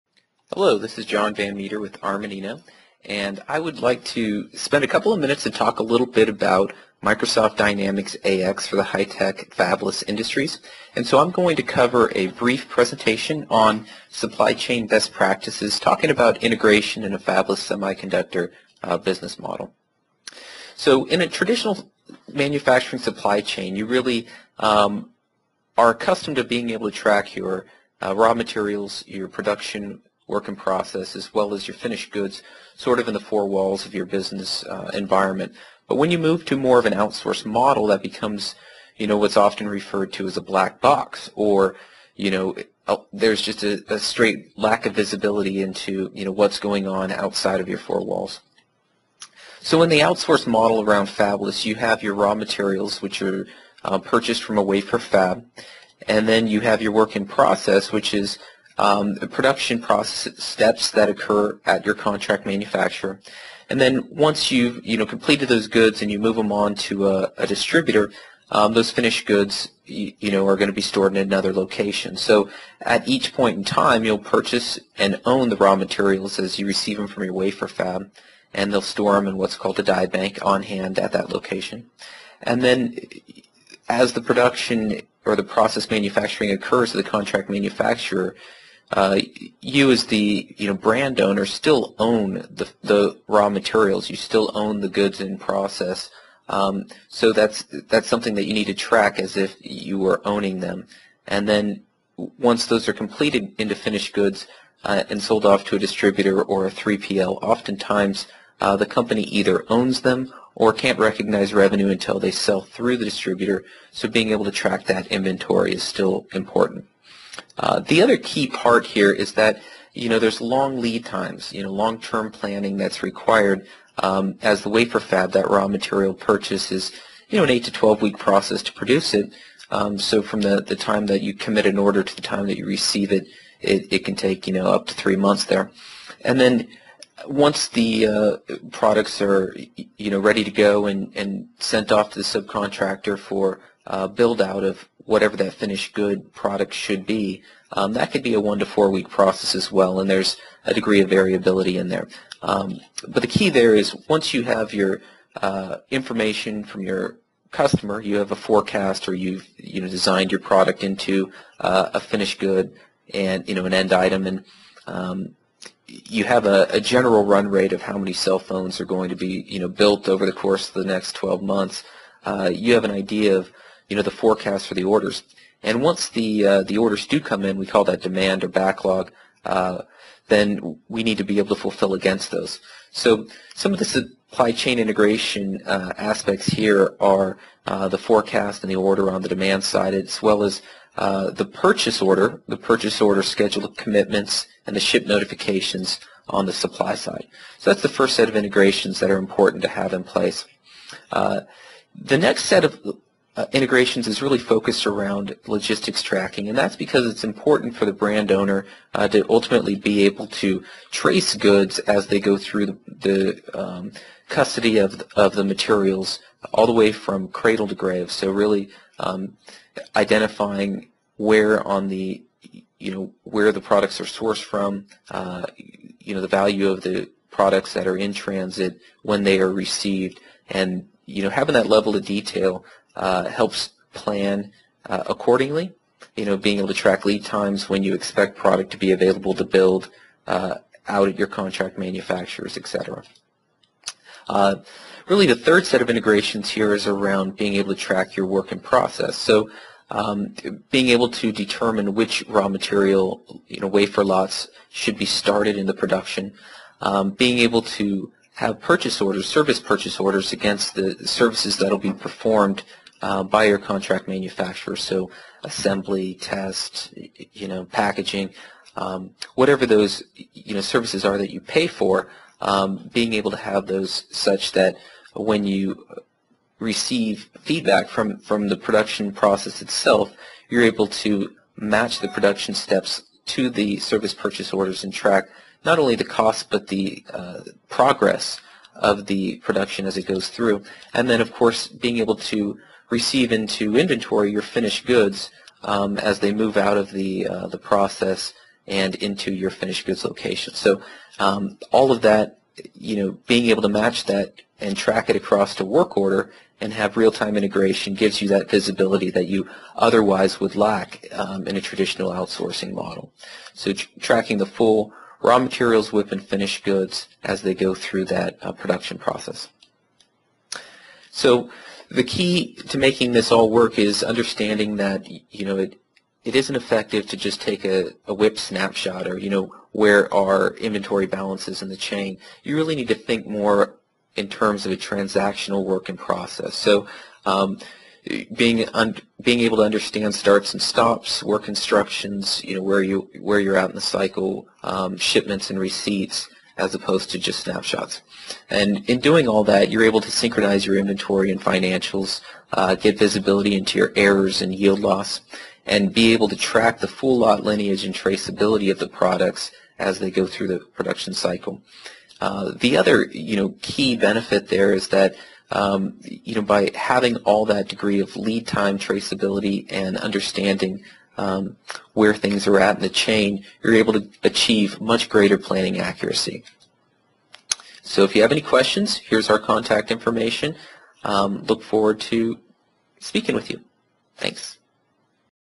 Managing a Fabless Semiconductor Supply Chain - Best Practices
For fabless outsourced semiconductor manufacturers, ERP integration is essential to improving lot lineage, traceability, logistics and tracking works-in-progress beyond just snapshots. Hear some best practices and learn the importance of three key ERP integrations and how they roll up into greater planning accuracy.